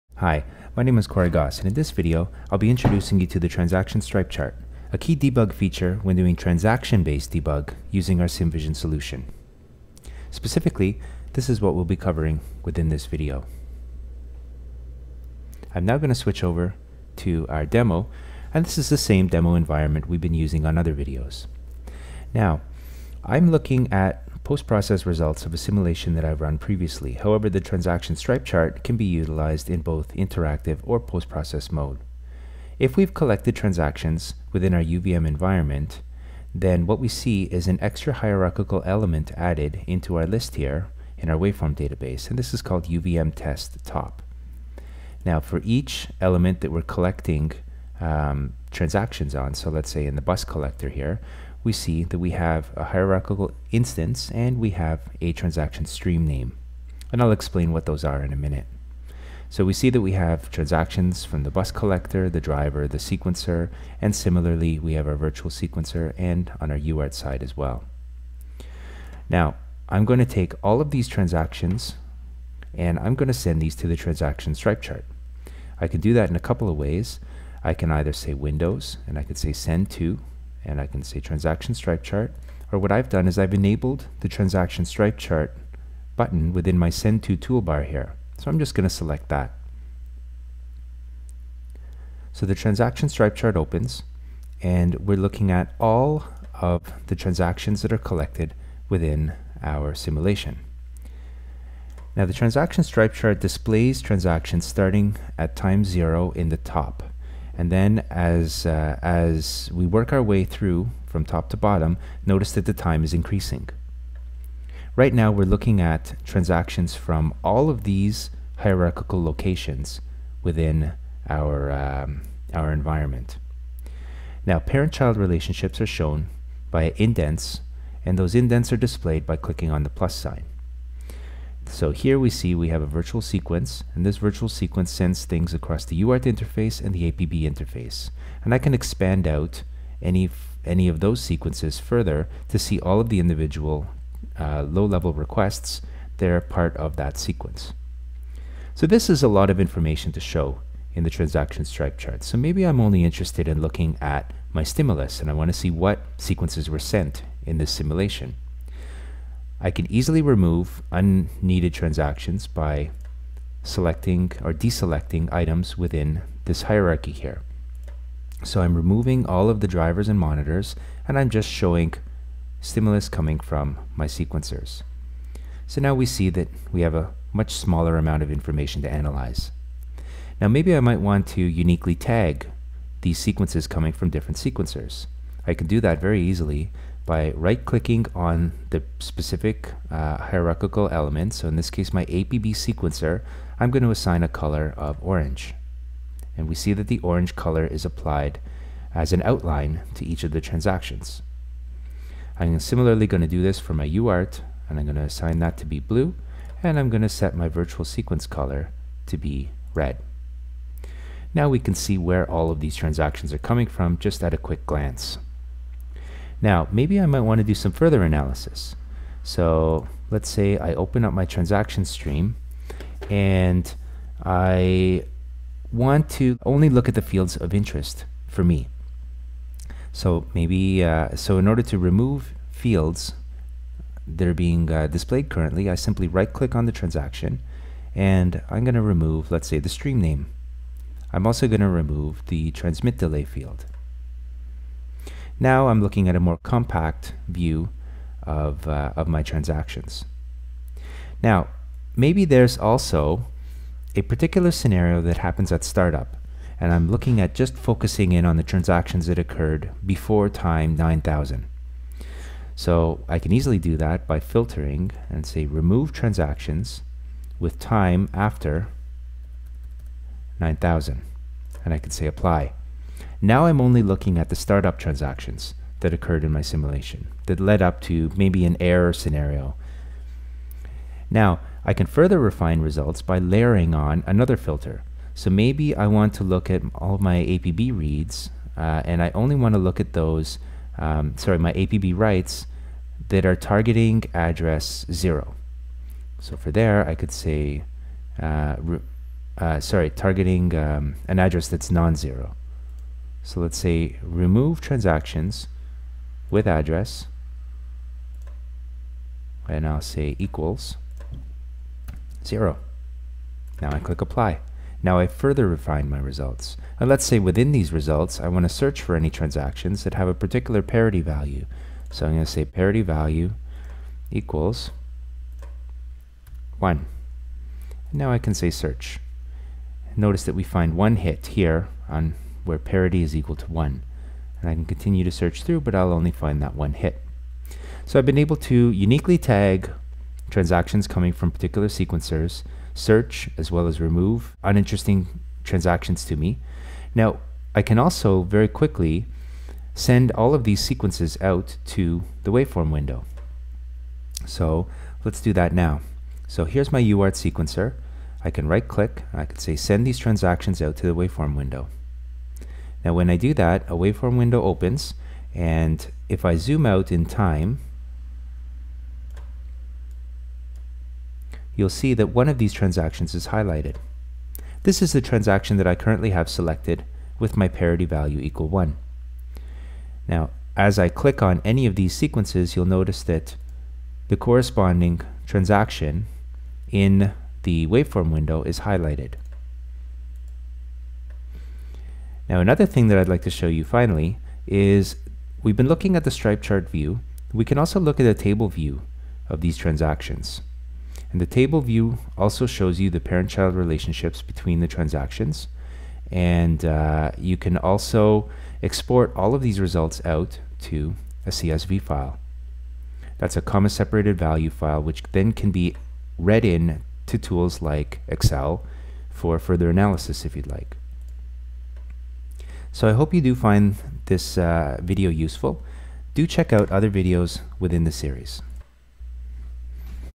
SimVision Transaction Stripe Chart Introduction
Quick introduction to the Transaction Stripe Chart within SimVision.  This window is very useful for displaying and analyzing transaction collected throughout the simulation.  Specifically, the videos focuses on transactions collected within UVM environments.  The video outlines how to send transactions to the stripe chart, how to select what transactions to display, how to view parent/child relationships, how to filter and search through transactions as well as add colour to specific transactions streams.  It also covers how to synchronize transactions with those displayed in the waveform and also how to export transactions to a CSV file for external analysis or sharing.  For more information on the probe commands required to collect transactions, see the video titled "SimVision Class and Transaction Debug (Post Process)".  Video is based on the 12.2 release of SimVision.

Note: This video was created using the 12.X releases of SimVision.